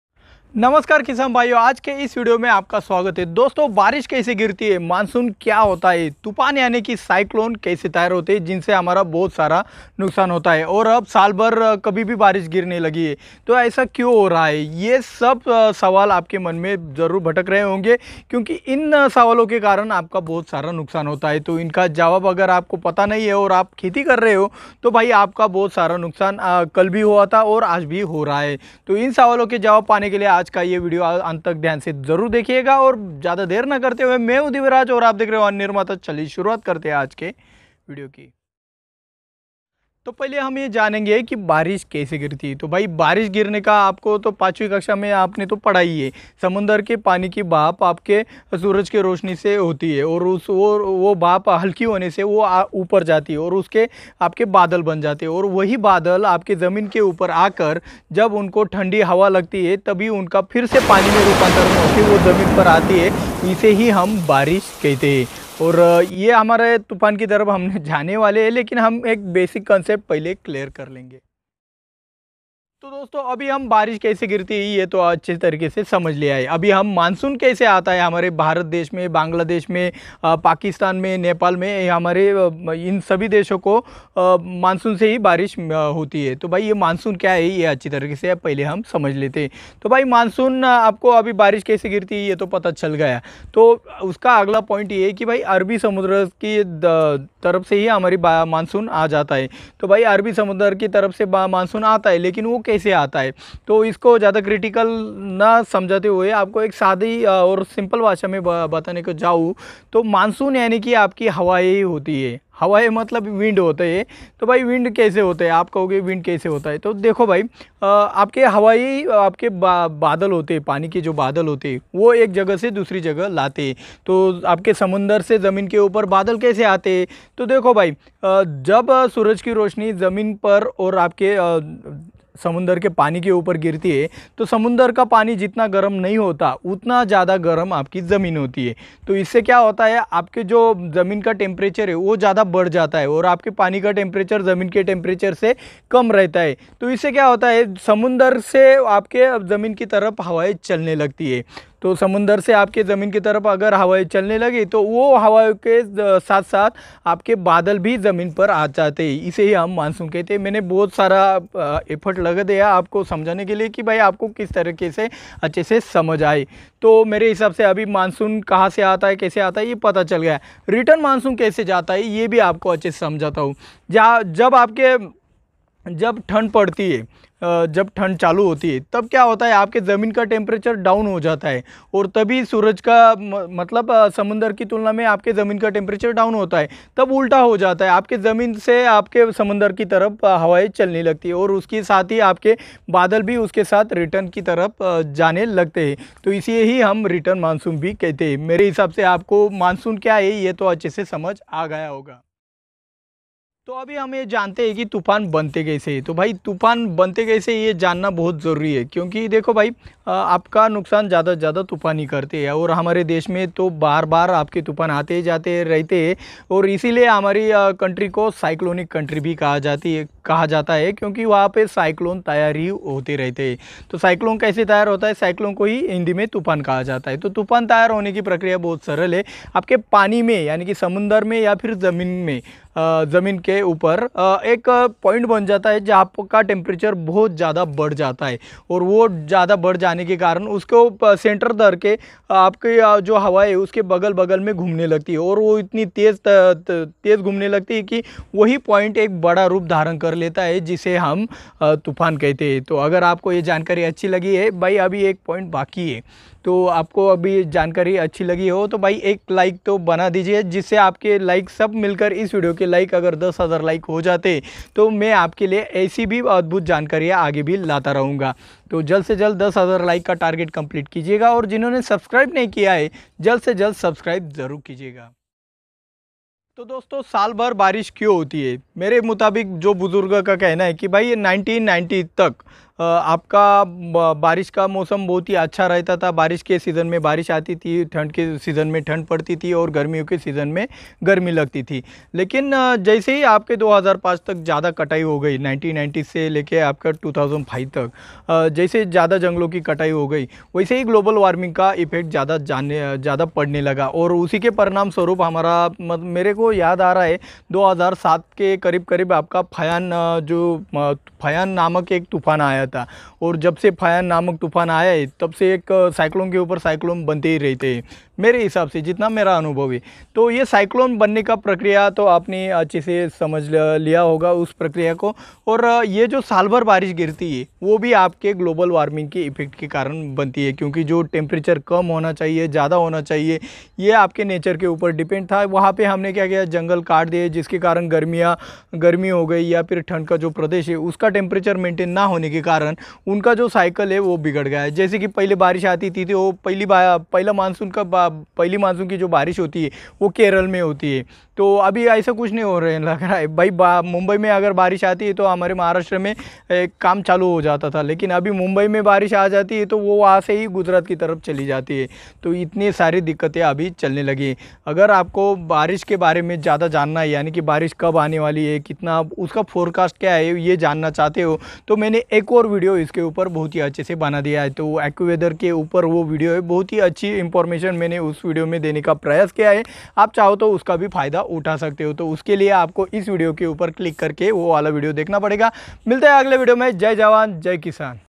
Cyclone/rain/thunderstorms/Mansoon
in this video we describe about following points,
cyclone,
rain,
thunderstorms,
agriculture,
mansoon,

00:00 तुफान और खेती
00:39 बारीश कैसे गिरती है?
01:11 मान्सुन कैसे आता है?
03:22 तुफान कैसे बनता है?
04:35 सालभर बारीश कैसे होती है?

created by :
divyaraj kotkar :
For support channel: phone pay id is ''   divyaraj777@ybl    ''